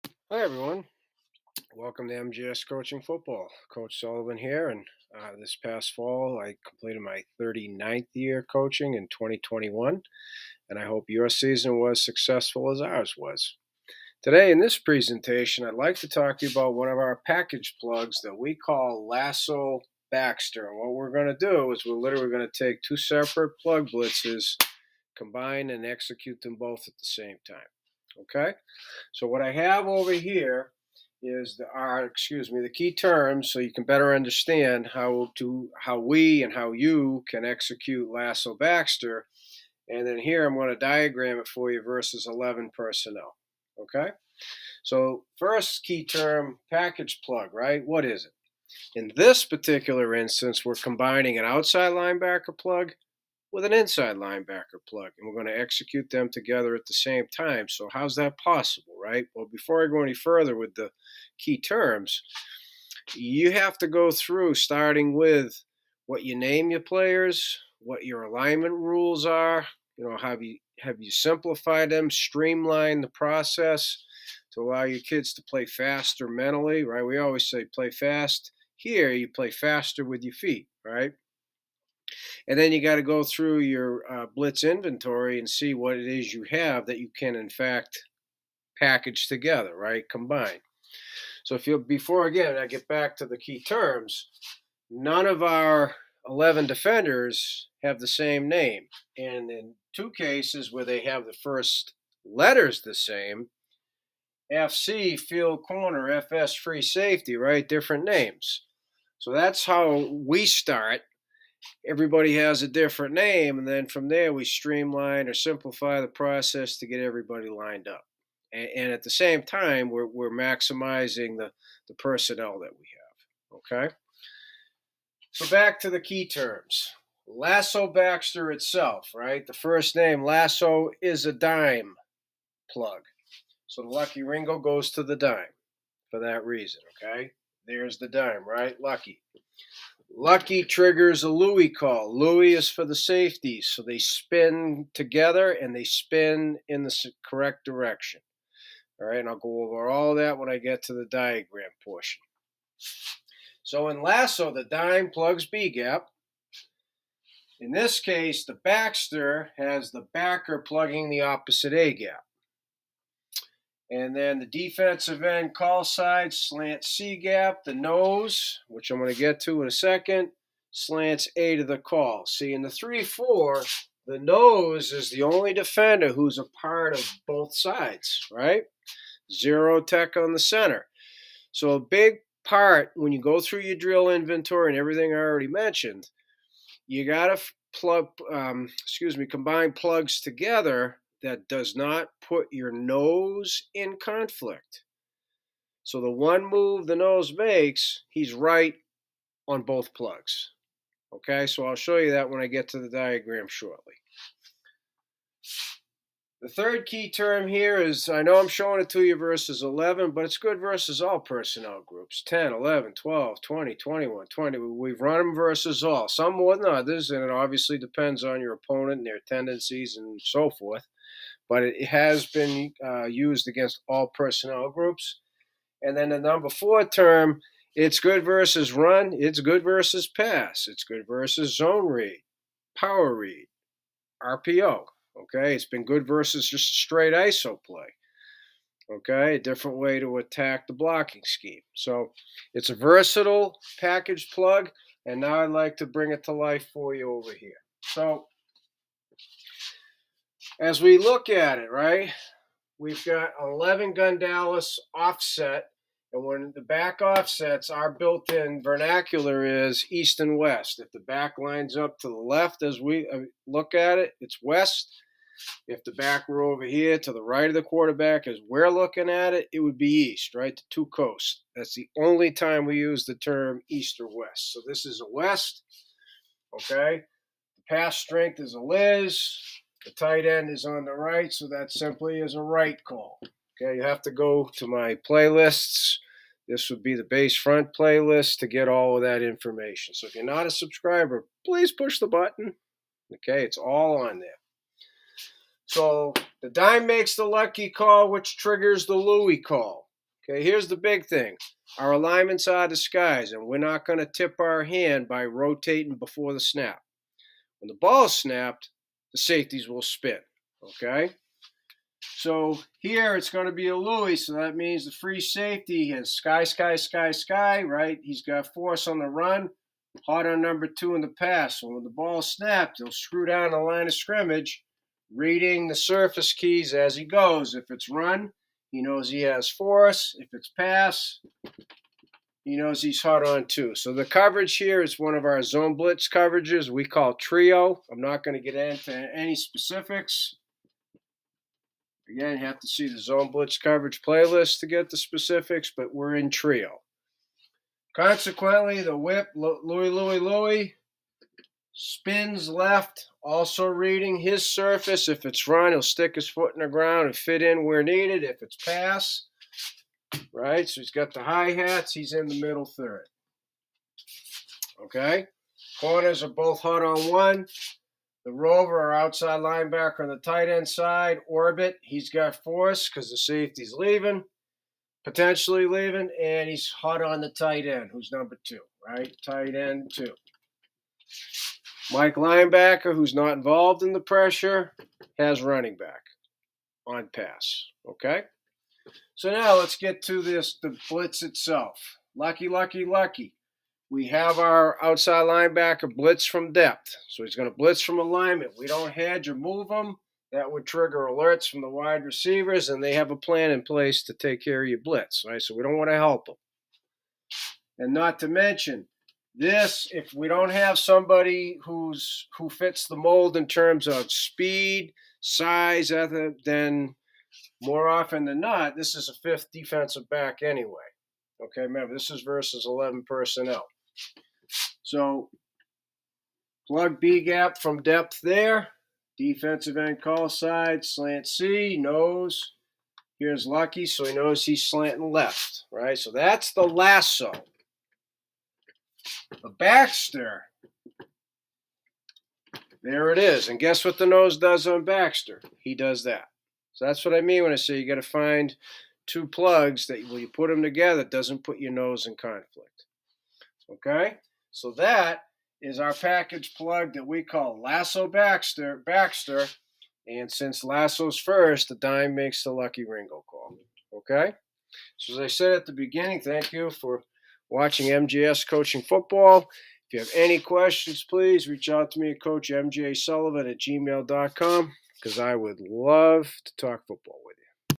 Lasso Baxter Packaged Plug vs. 11 per.
Learn how we combine OLB and ILB plugs into one effective Packaged Plug pressure.